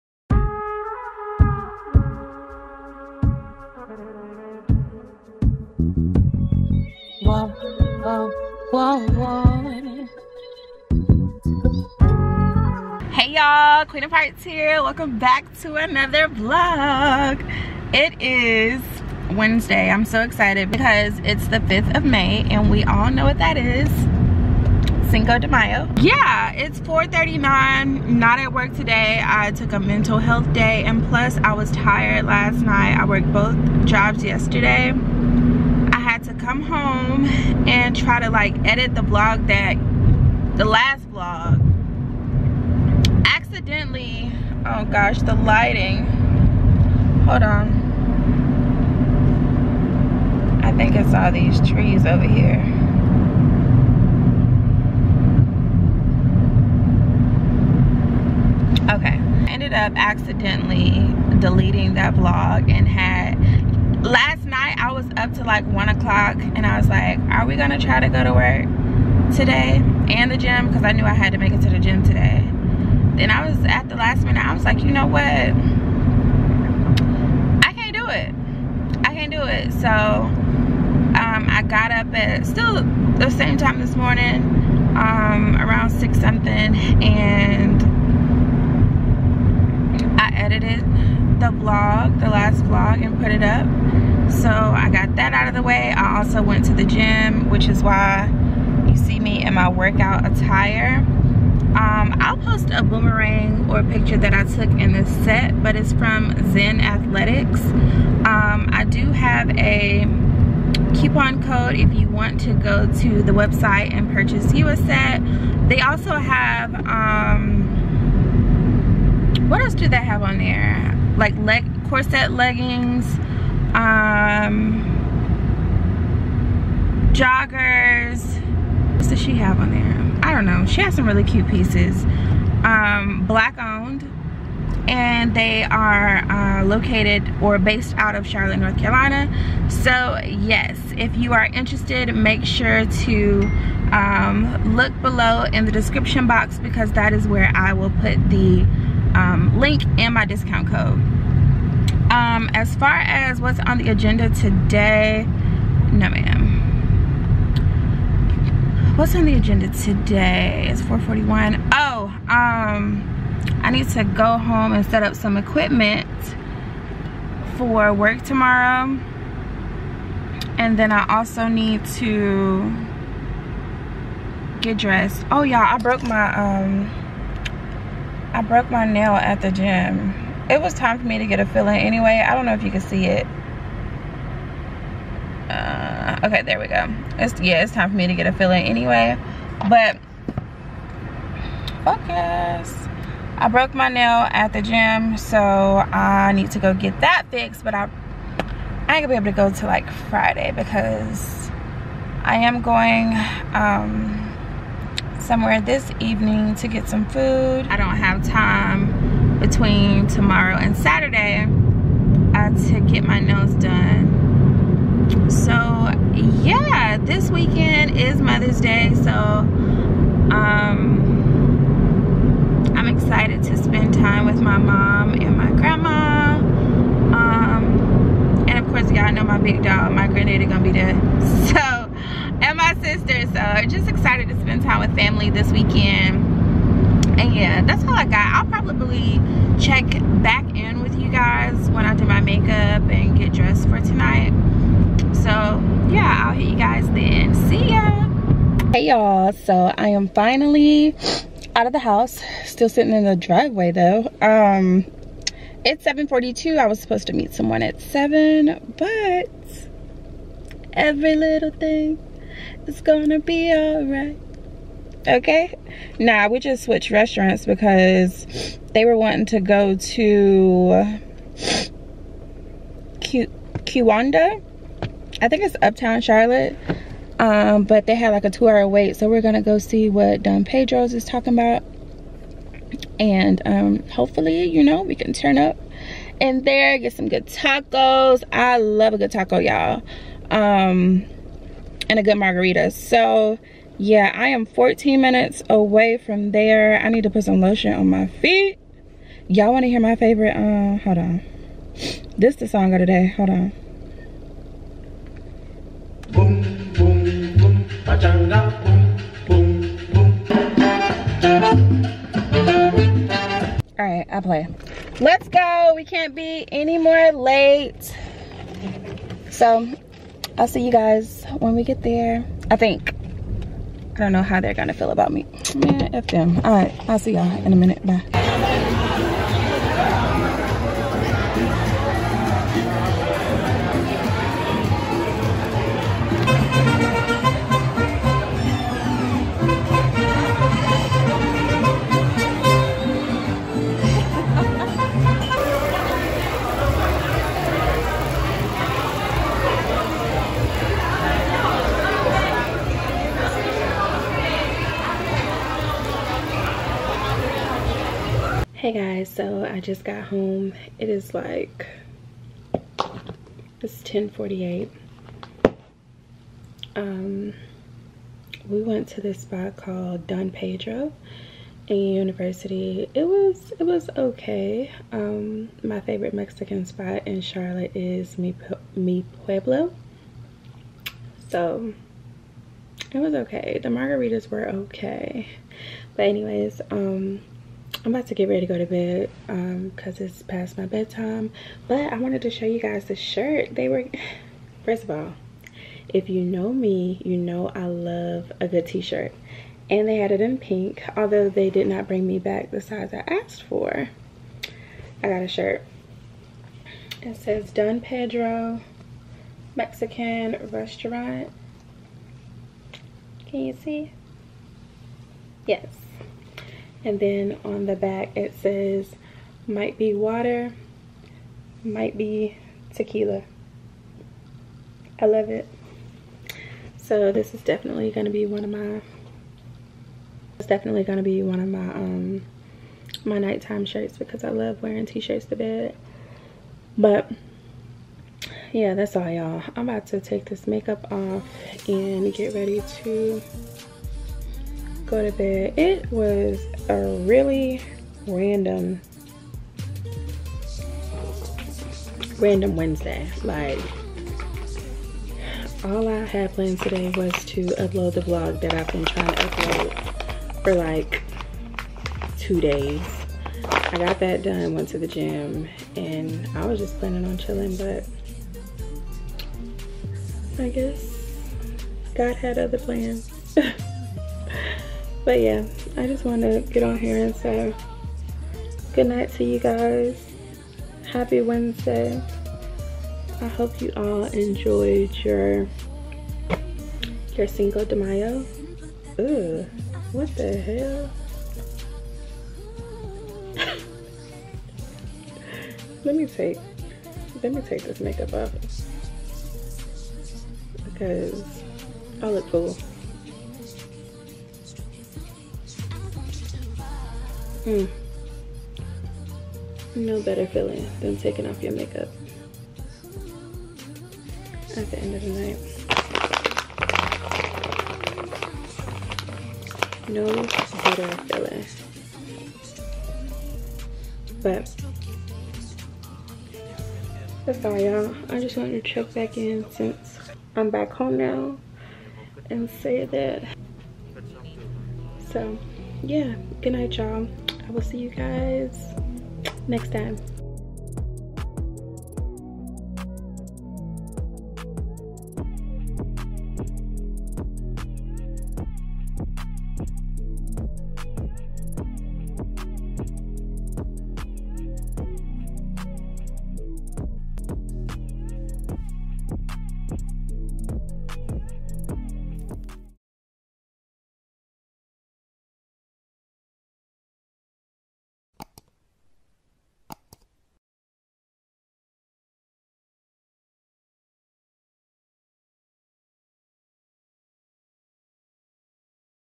DAY IN THE LIFE | CINCO DE MAYO VLOG |CHARLOTTE NC|QUEENOVHEARTS
Hey yall, so yeah. Todays vlog is super random lol. Thought I'd take you all along with me on a daily whatchamacallit. I hope you all enjoy this vlog. What did you do for Cinco De Mayo?
F O L L O W   M E   O N   I G : @_queenovhearts
Subbie Count:1,634
Code “QUEENOVHEARTS”